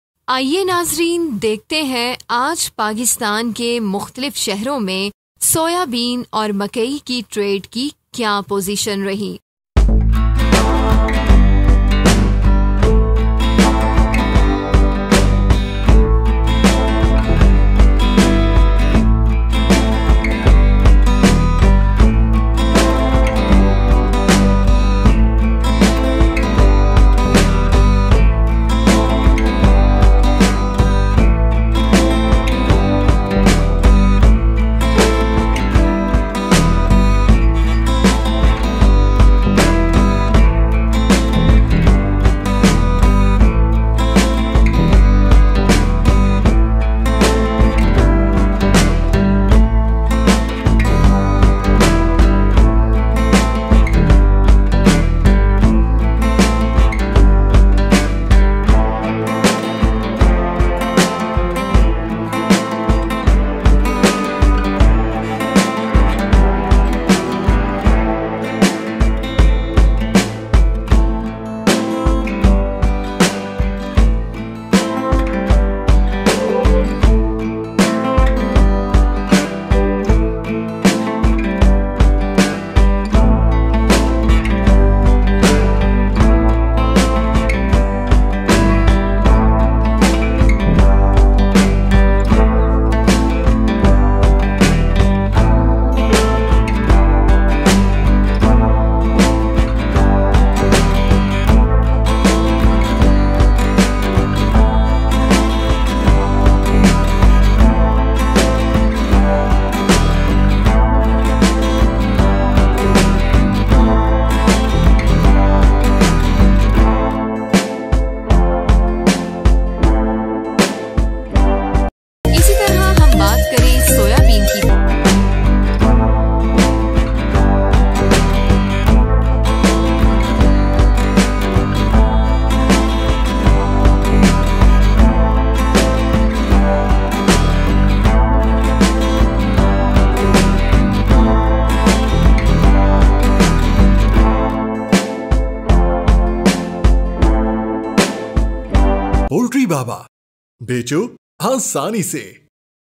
Today Corn and Soya bean Price in Ghala Mandi |Maize Rates In Pakistan Poultry Baba .
Today Corn and Soya bean Price in Ghala Mandi  |  Maize Rates In Pakistan | Poultry Baba  | poultry farm business plan.

Today Corn and Soybean Prices in different cities of Pakistan – Corn and Soya Bean rates
Choose POULTRY BABA

Corn (Maize)
Maize is being grown on an area of 1.016 million hectares with an annual production of 3.037 million tons and an average grain yield of 2,864 kg/ha Maize production in Pakistan has increased from 0.38 million tons during 1947-to 50 to 3.037 million tons in 2007.
In Pakistan, the maize crop is sown mainly in two seasons; spring and autumn season. Spring maize can be planted in the first week of February up to the first week of March while the sowing time for autumn maize starts from the last week of July and ends in mid of August. The seed rate for maize is 20-25 kg per hectare.

Soya bean
Soybean or soya bean may be a species of legume native to East Asia. Soya bean contains multiple vitamins such as Phytic acid, dietary minerals, and B vitamins. Soybeans are about 18% oil and 38% protein. Because soybeans are high in protein, they're a serious ingredient in livestock feed.
Potential Areas for Soybean;
PUNJAB: Rawalpindi, Attock, Jhelum, Chakwal, Gujrat, Gujranwala, Sialkot, Daska,  Sheikhupura, Lahore, Kasur, Sahiwal, Okara, Vehari, Khaniwal, Multan, Malsi
SINDH: Nawabshah, Hyderabad, Sangar, Tharpaker, Badin, Thatta
NWFP: Peshawar, Noshera, Mardan, Swabi, Malakand, Dir, Swat, Charsada, Mansehra, Kohistan
BALUCHISTAN: Sibi, Kachi, Kalat, Khuzdar
A smaller percentage is processed for human consumption and made into products including soy milk, soy flour, soy protein, tofu and lots of retail food products. Soybeans also are utilized in many non-food such as industrial products.